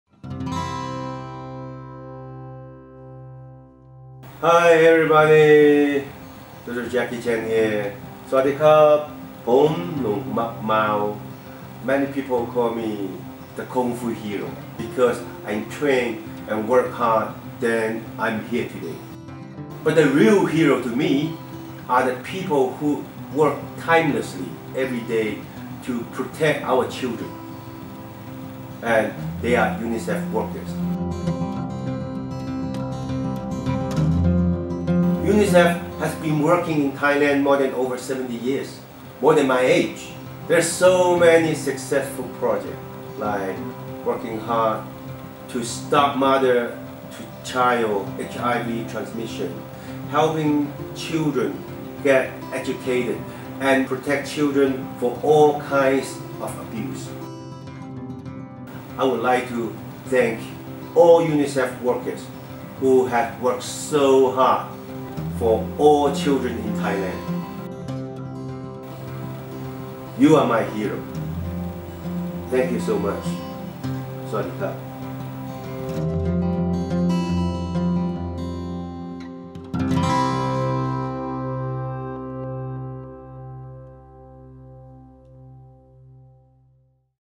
What does Jackie Chan, the 'Kung Fu Hero', mean by the 'heroes for children'?
When we say the Drunken Master, we can only think of Jackie Chan. With his action packed performance, many people call him the “Kung Fu Hero”.

As the Kung Fu Hero joins the mission for children, he meets ‘the children’s heroes’ but who are they?

Let’s hear from Jackie Chan.